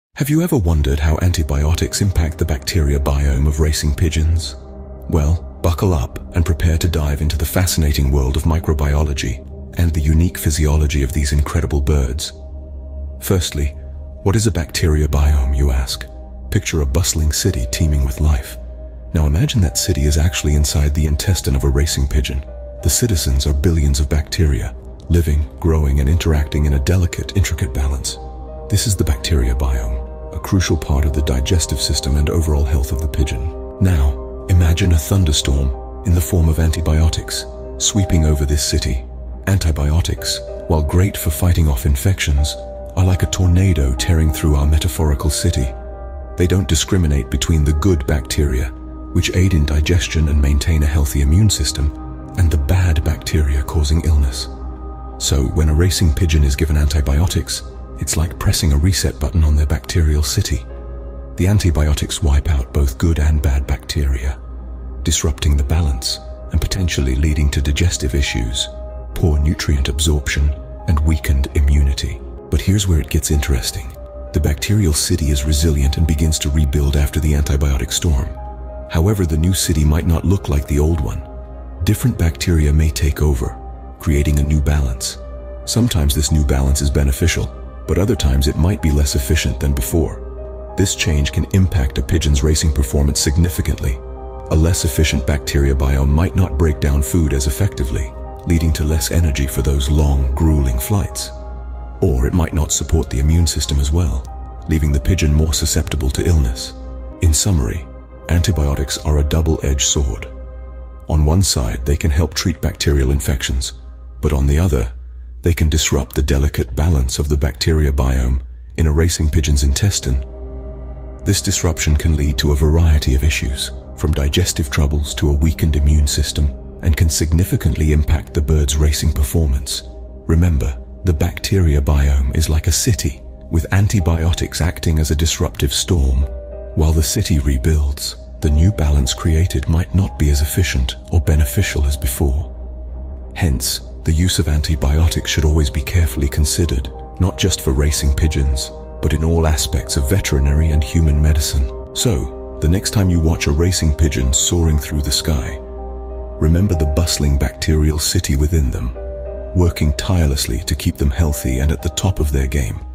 The Antibiotics and Bacteria Biome Connection in Racing Pigeons
In this video learn why Antibiotics can have life long negative effects on our racing pigeons.  You can decide to have stronger or weaker pigeons.